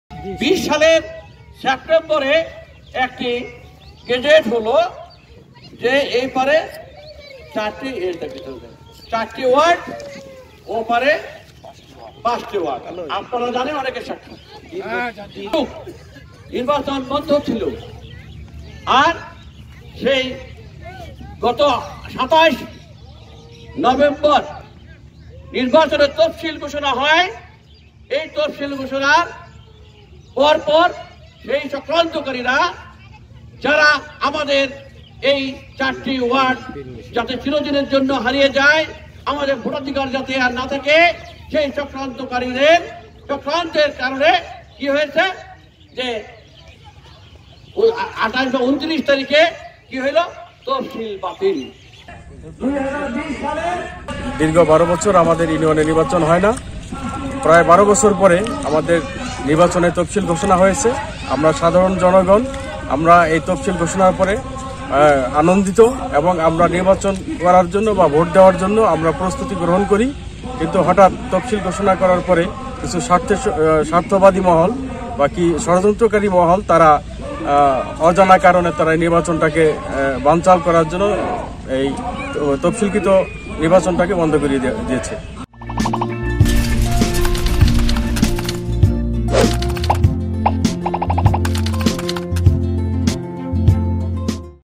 তফসিলের পরও নির্বাচন বাতিল হওয়ায় ক্ষুব্ধ ইউনিয়নবাসী || Jago News
তফসিলের পরও নির্বাচন বাতিল হওয়ায় ক্ষুব্ধ ইউনিয়নবাসী